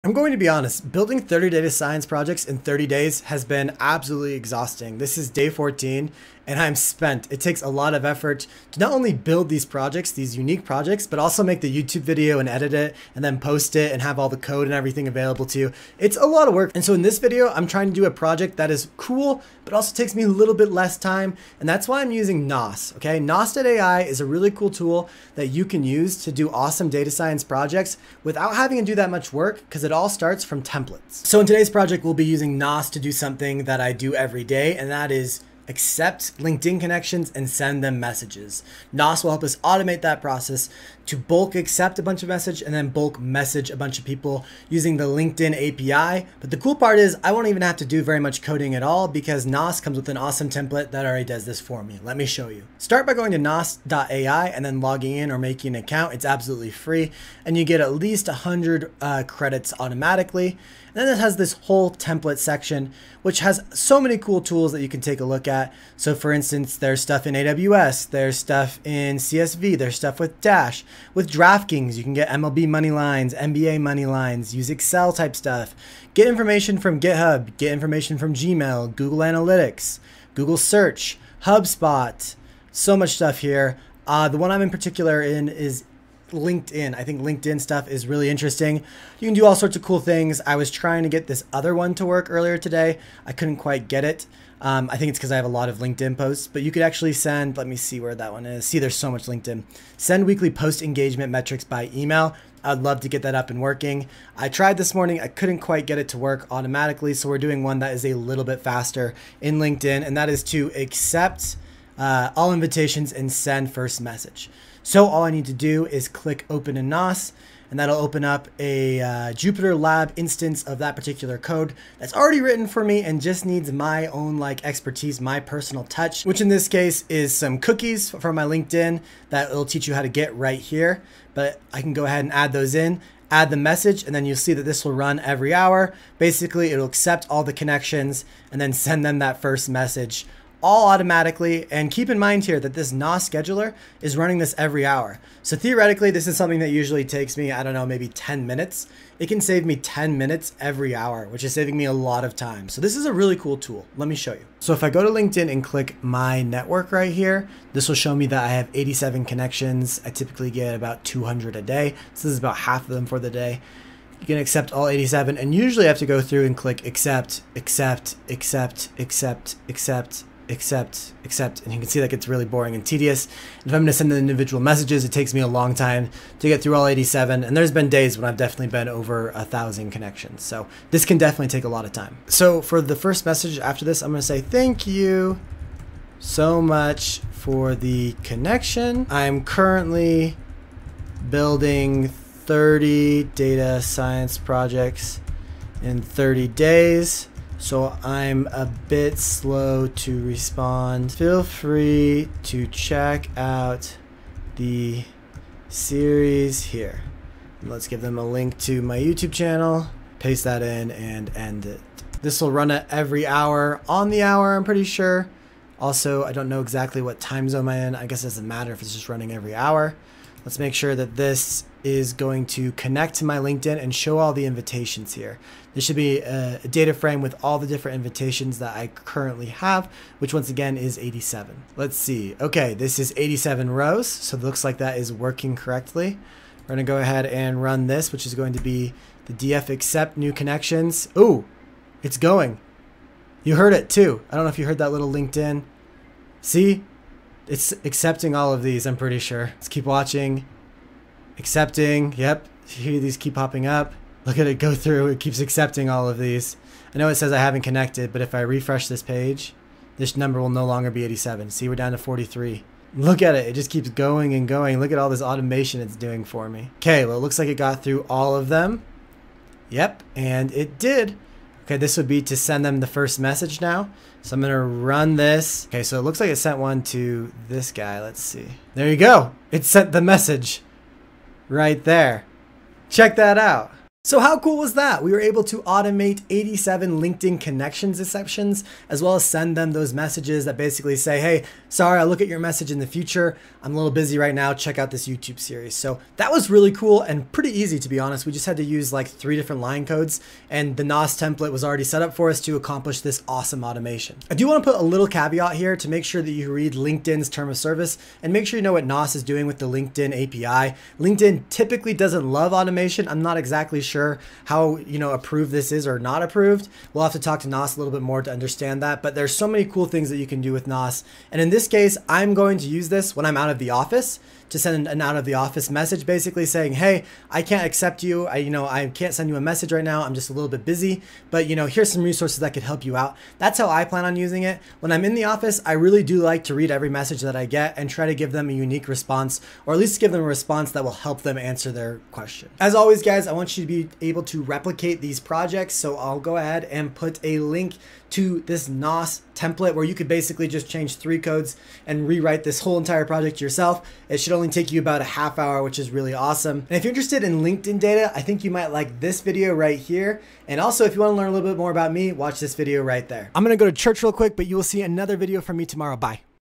Automate LinkedIn Using Python 🤔
Ever get tired of accepting LinkedIn connections? When you get hundreds a day, it can be time consuming to press accept and message each person individually. In this video, we will use Python to fast-forward the LinkedIn connection request, and first message send with the help of naas.ai

This is Day 14 of building 30 data science projects in 30 days!

Also, LinkedIn if you're watching, this is for demonstration purposes only.

See you tomorrow PROJECT DOERS!